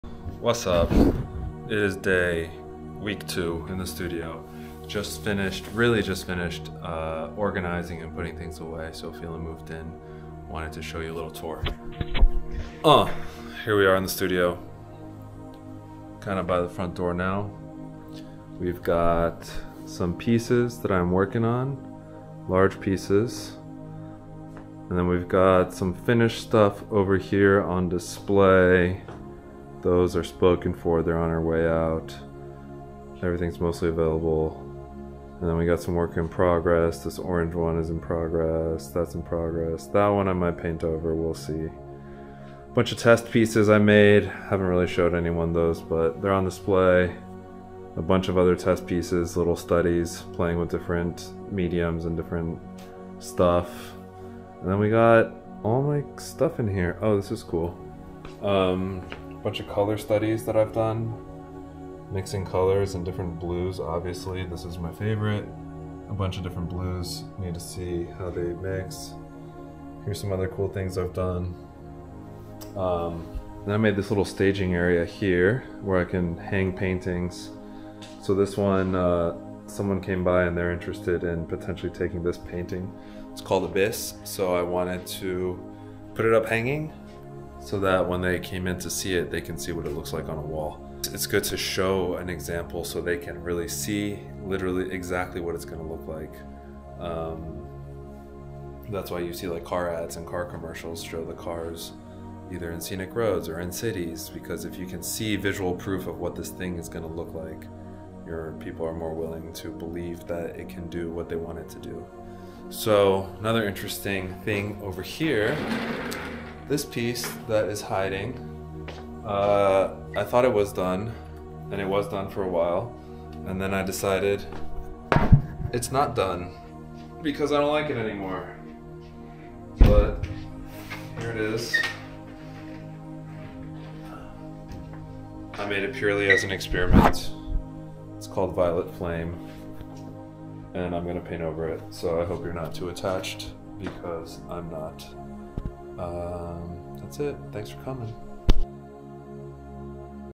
Professional Art Studio Tour + Unreleased Paintings
In this video... a peak inside my brand new art studio, including some unreleased paintings, some new works in progress, and more.

to see my art and to join my insider list.